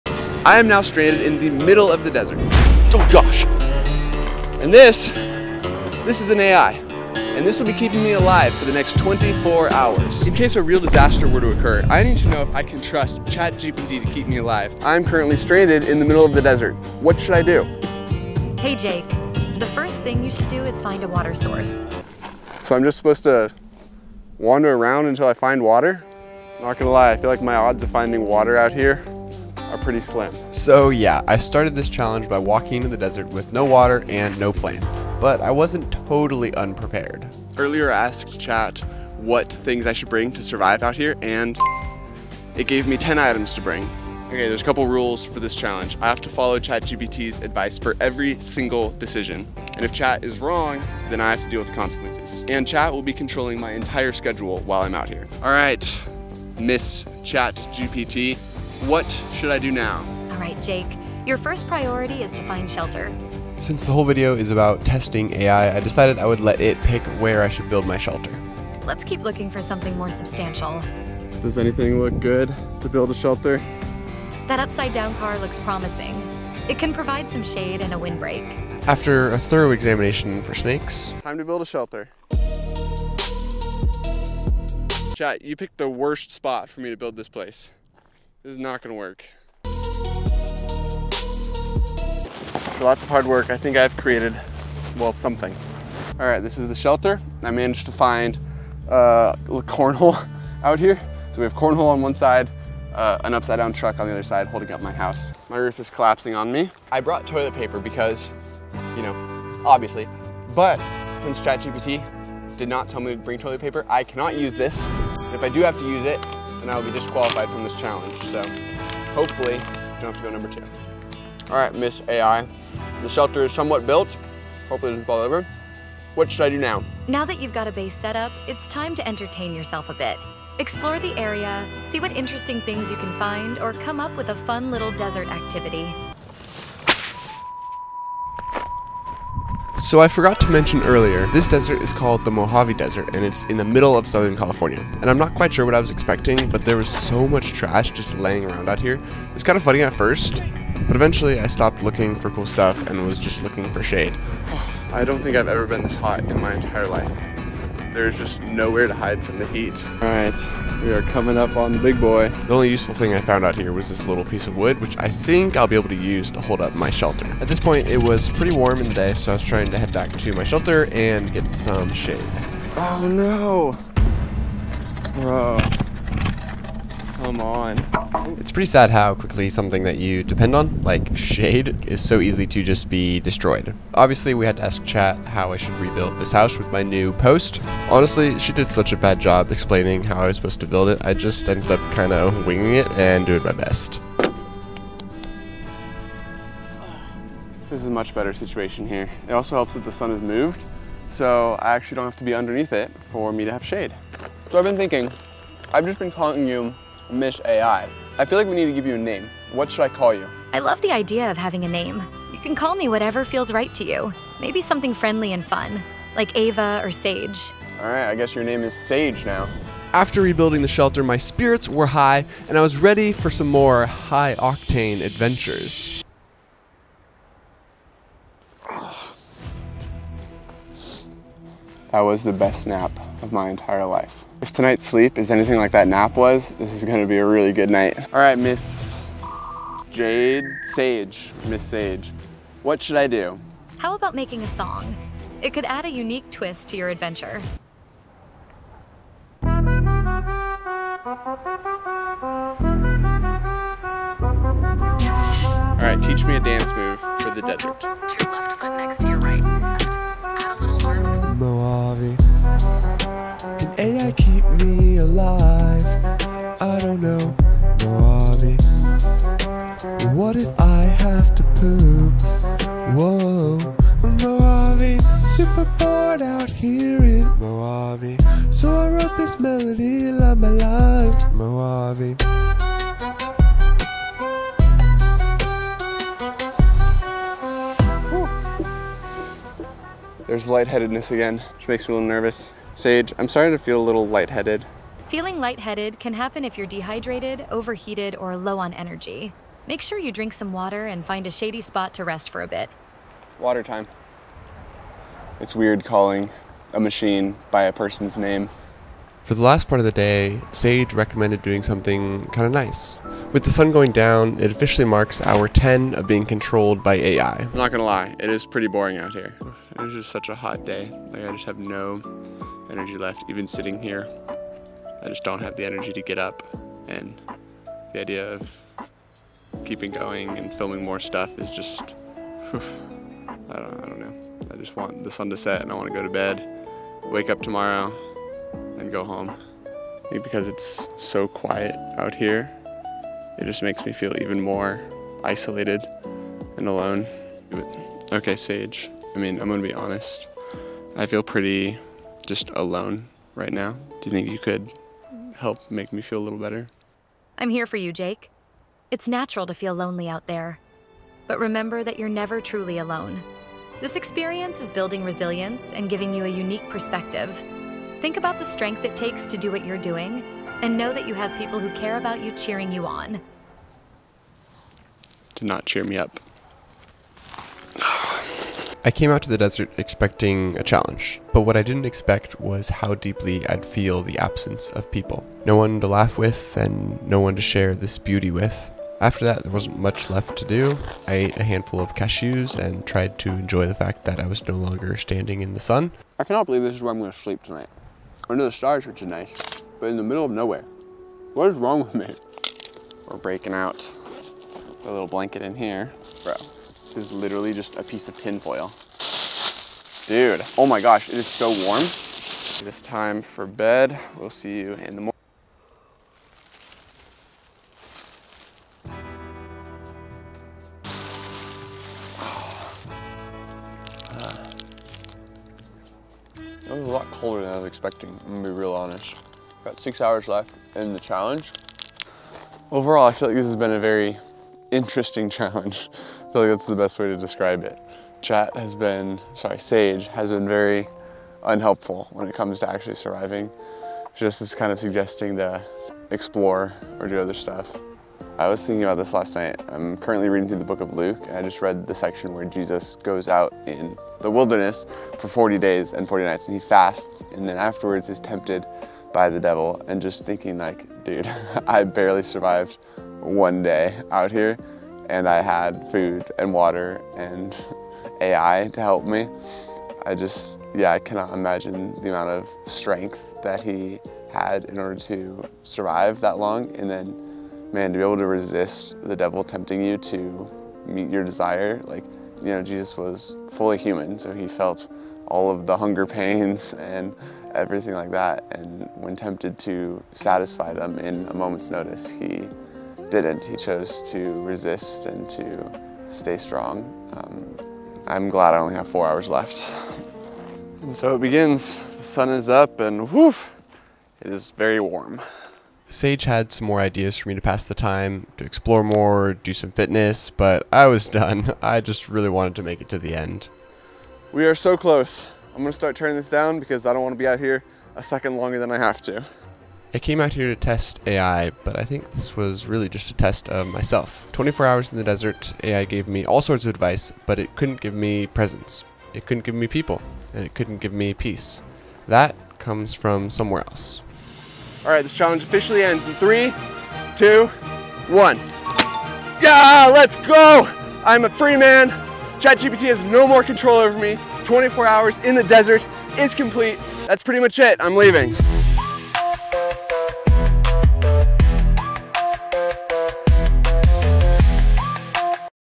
Can ChatGPT help me survive in the desert?
I’m in the middle of the desert. No map, no plan — just an AI telling me what to do to survive. For 24 hours, I gave up control and let artificial intelligence make every decision: where to go, what to eat, how to find water, and when to rest. What could go wrong?

This wasn’t just a survival challenge — it was a full-on experiment in trusting technology when everything’s on the line. Can AI actually keep a human alive in one of the harshest environments on Earth??